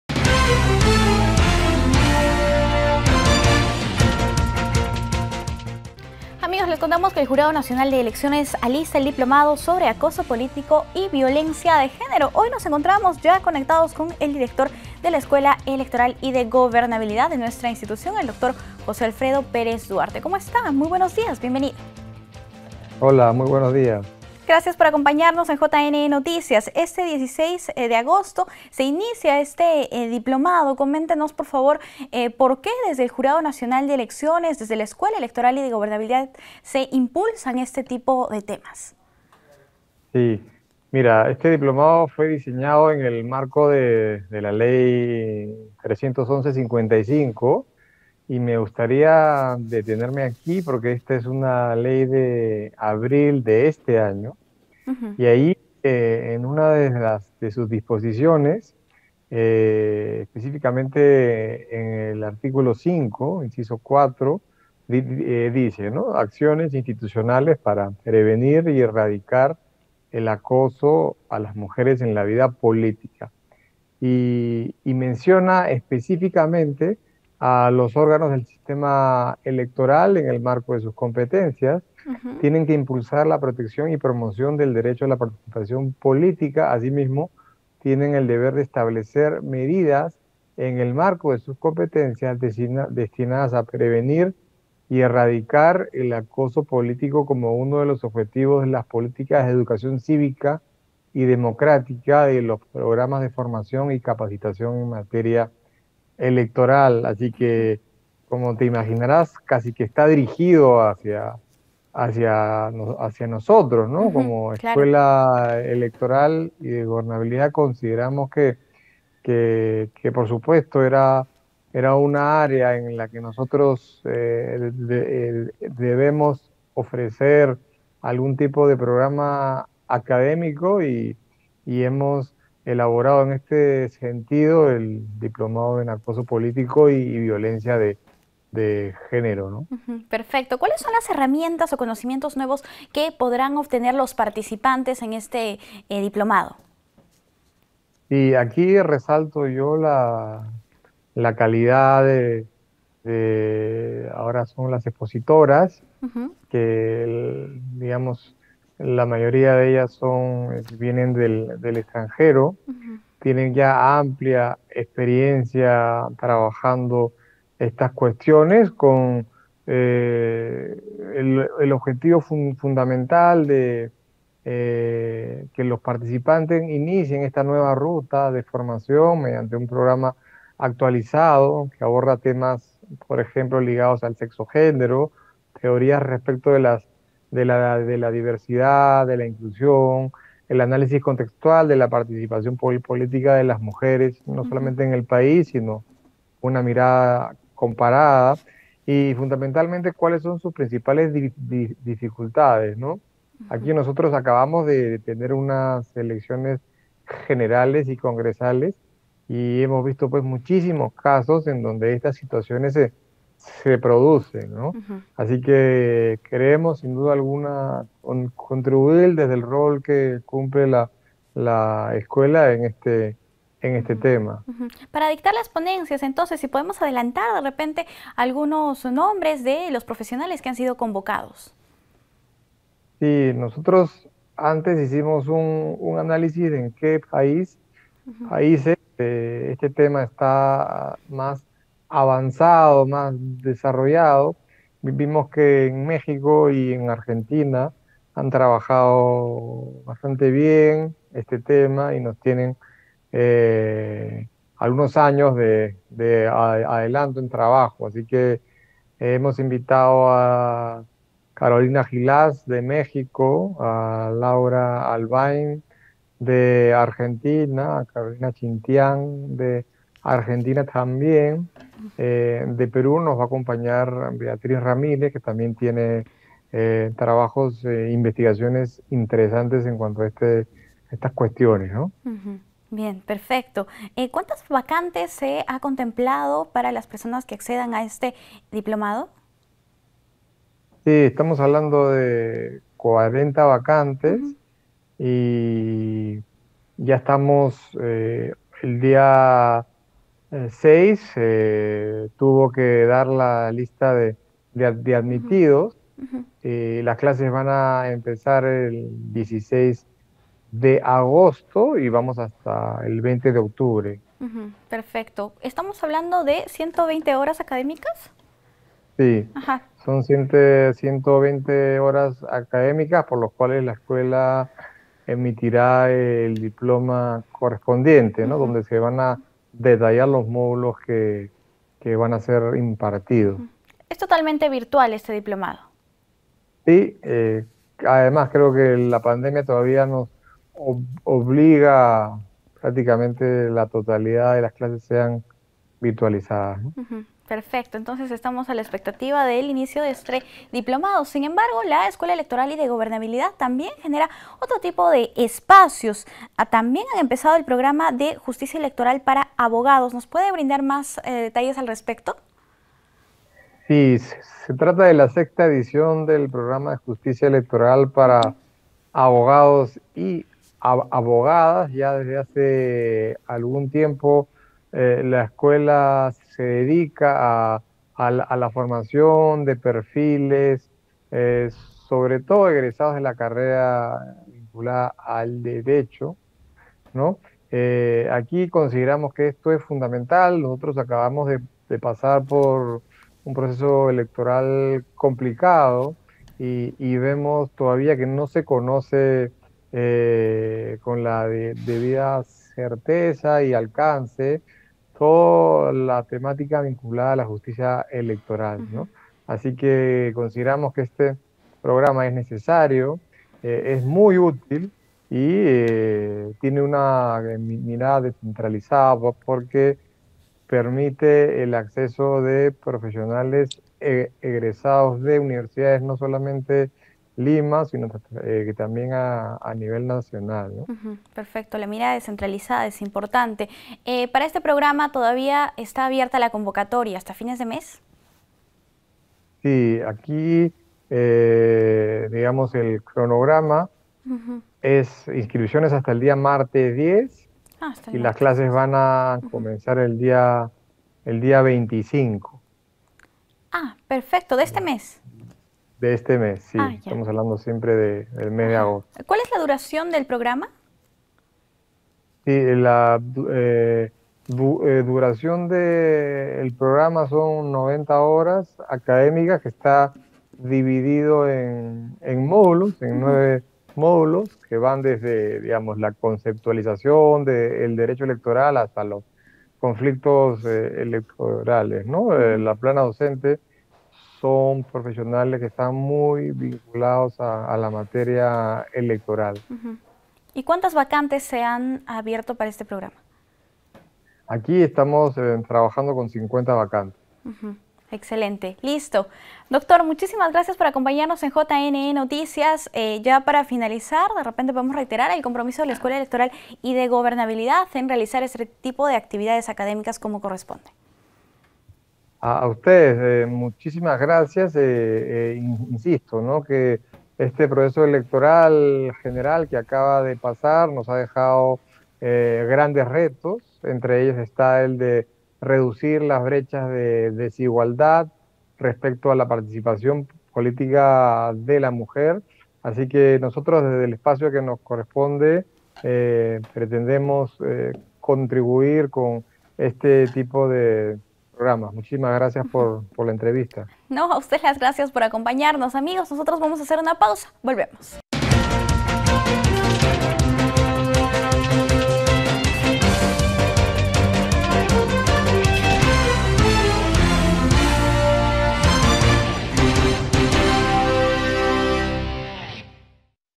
JNE ALISTA DIPLOMADO SOBRE ACOSO POLÍTICO Y VIOLENCIA DE GÉNERO
JOSÉ ALFREDO PÉREZ DUHARTE, DIRECTOR DE LA ESEG